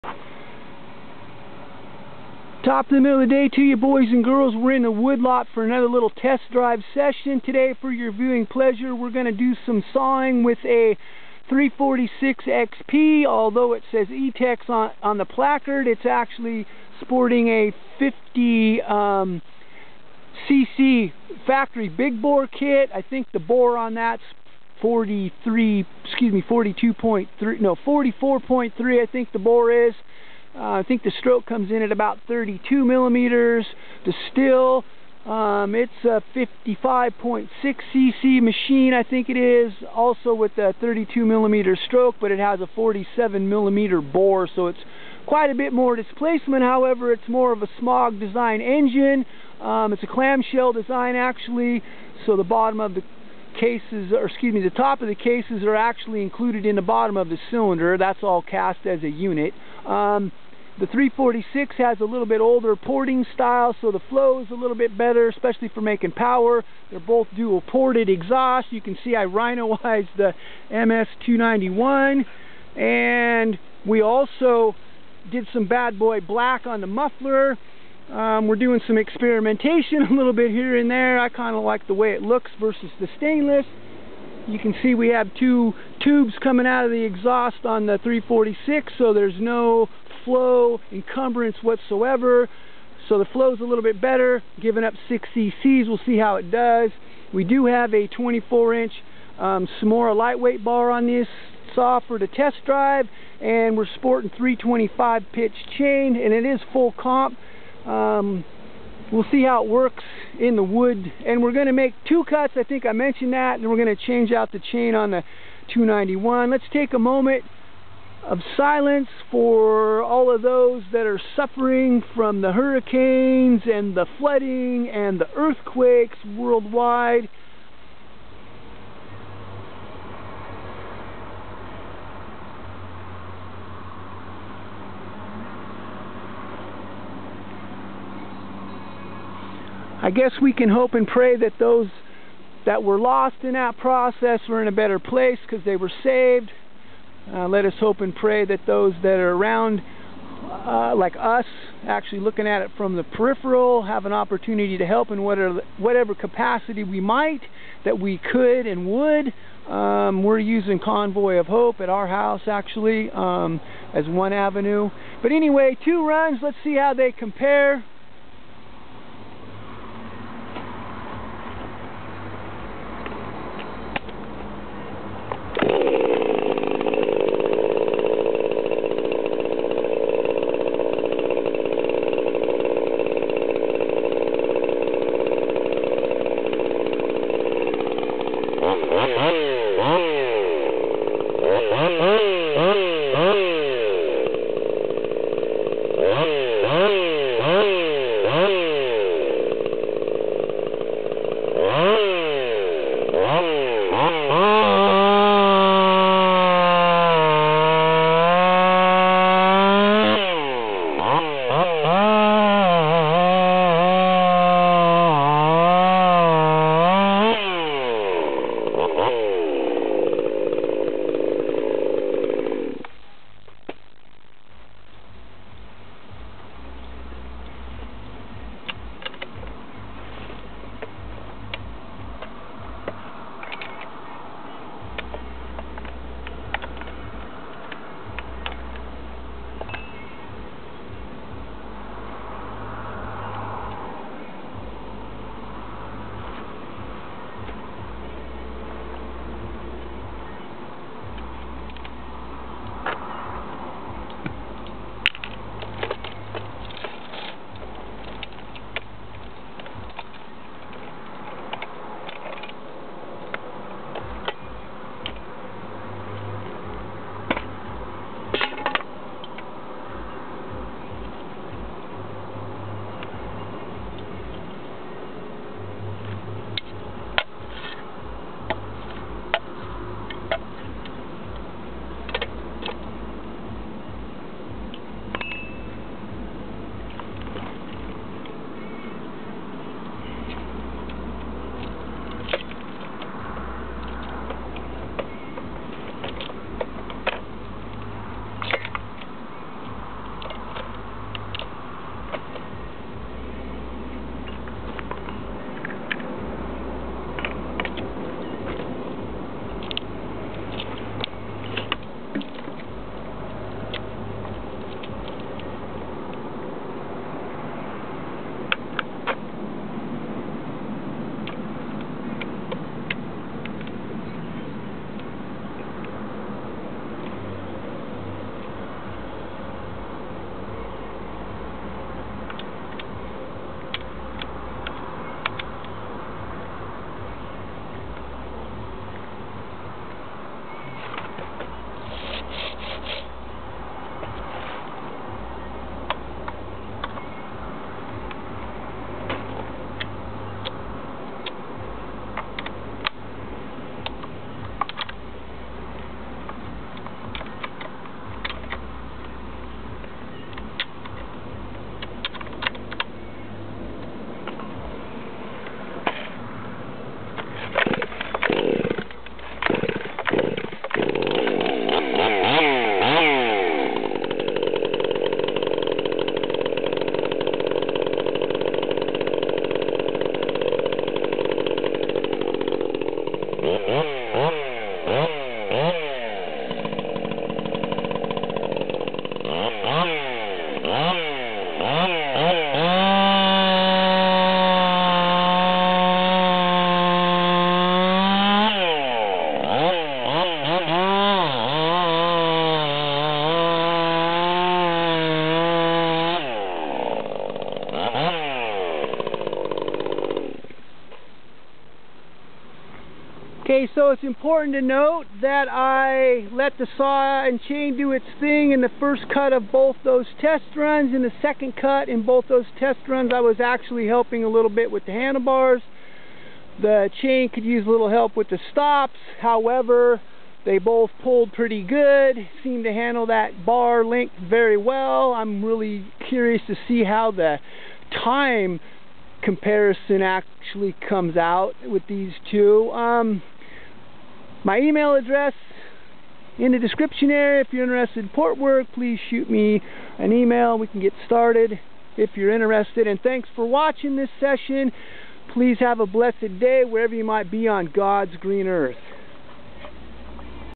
Husqvarna 346xp vs. Stihl ms291
First test drive(s) on a new Stihl ms291 (clamshell design cylinder, coming out of Northern Cali)) with the insane-o zipkit and some combustion chamber work versus a gently used Husqvarna 346xp (hails from Tennessee) also with the insane-o zipkit and some combustion work as well.
Both were test run using a Tsumara 24" bar and .325 chain.
I thought it was a good comparison since the Stihl has quite a bit more displacement but is more of a smog design engine. Turns out the timed runs were pretty much the exact same.
I am currently available for port work. If you are interested shoot me an email and we can start the process.